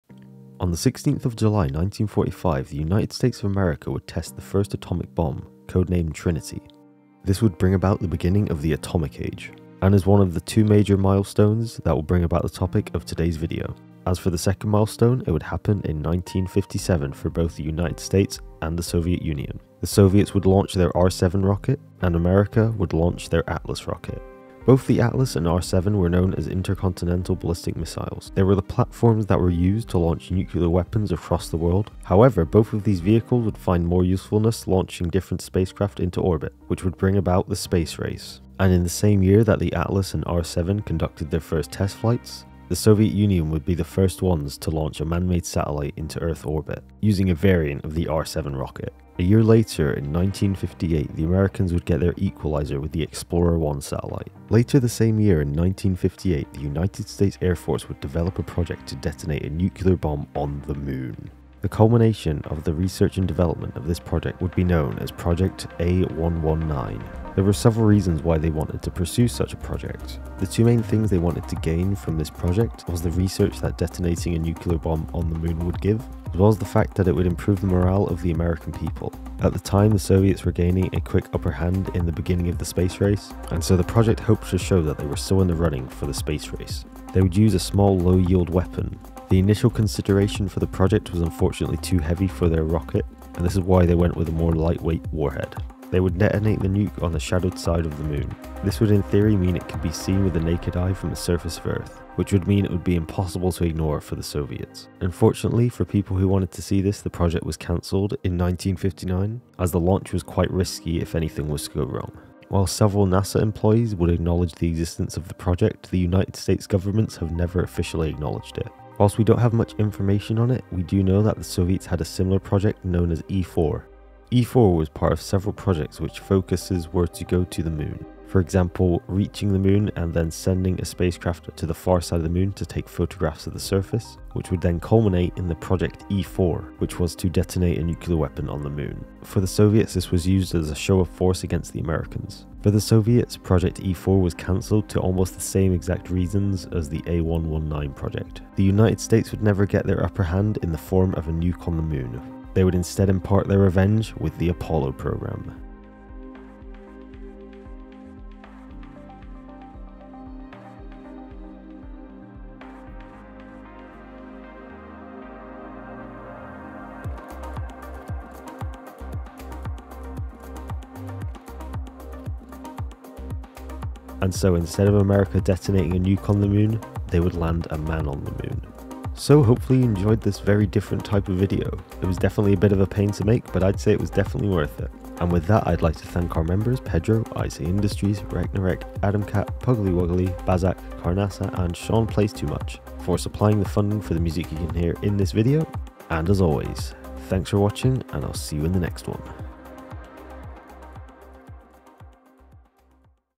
We Almost NUKED the moon... Project A119 / E-4
In this video we explore Projects A119 and E-4, the American and Soviet proposals to drop a nuclear weapon on the moon...

for modifying their craft to be a nuke for me!

[Music]

Our Second Channel: DeepSpaceIndustries 2.0 - YouTube

Edited: Cameron._.
Recorded: Cameron._.